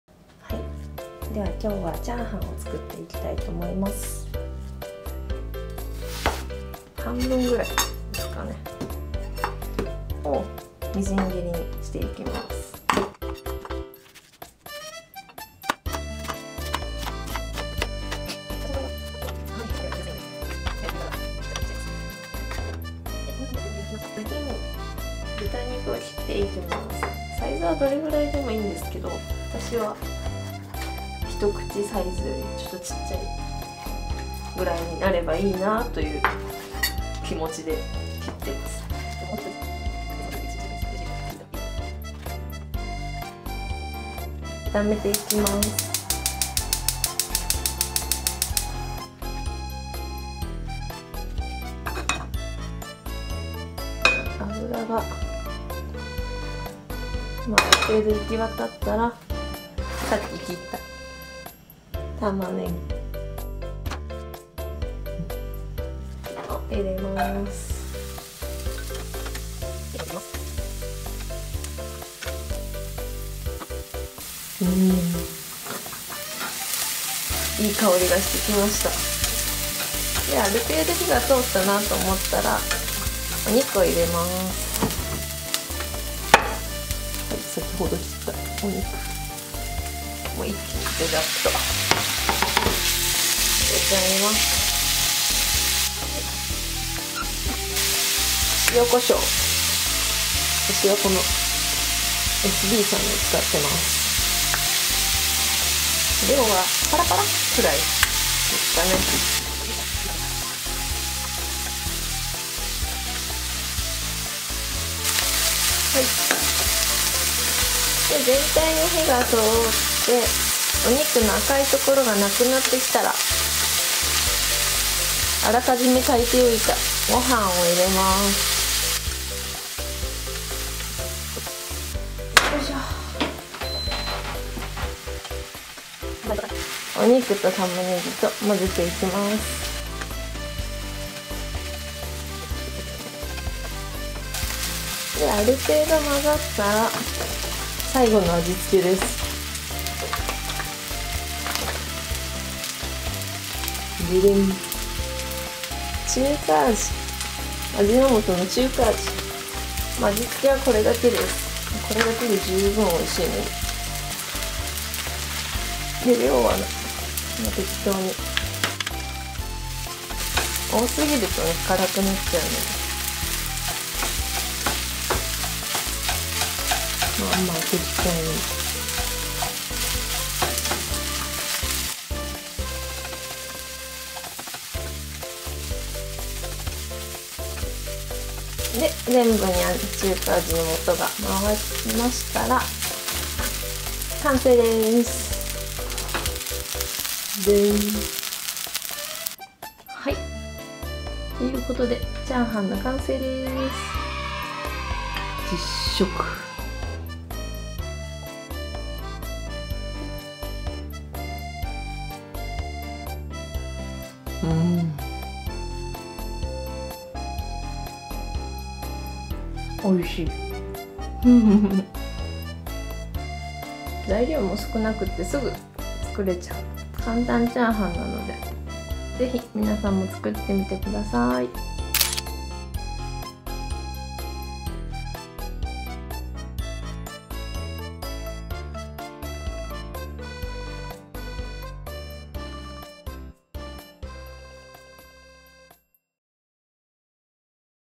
How to cook "Fried rice"!
1. Cut the onion and the pork
2. Stir-fry the onion, the pork and rice with salt, pepper and Chinese stock